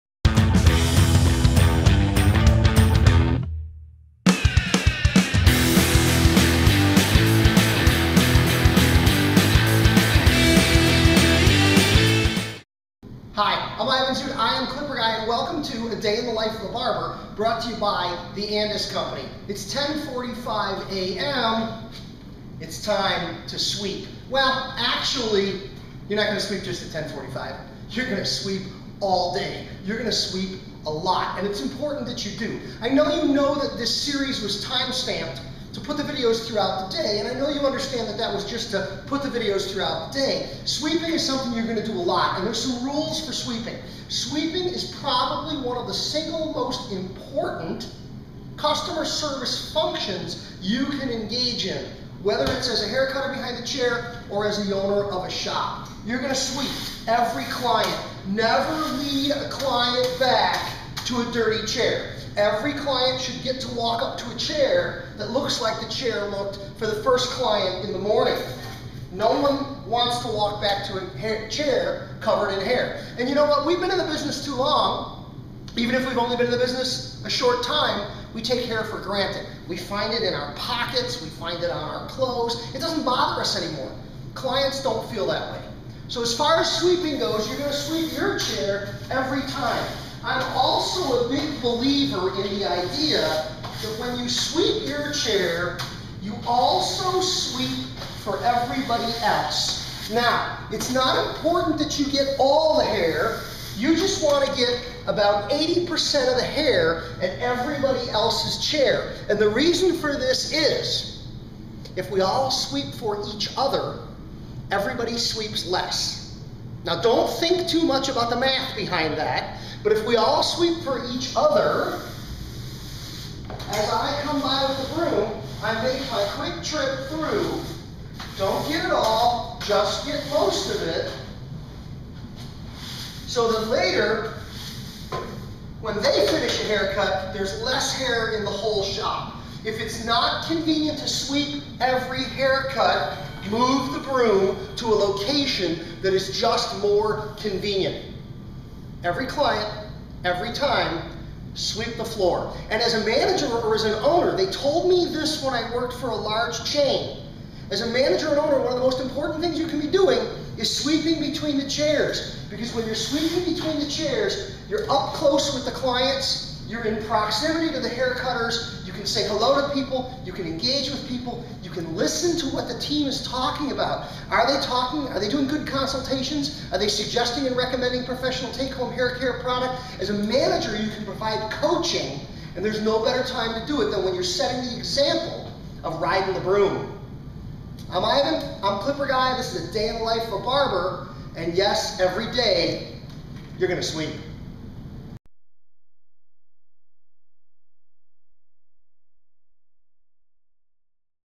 Sweep the floor 10:45 am - A day in the life of a barber shop
Welcome to A Day In The Life Of A Barber.
Featuring Ivan Zoot AKA ClipperGuy
Subject - Sweeping the floor in a barber shop is both a sanitation and a management function.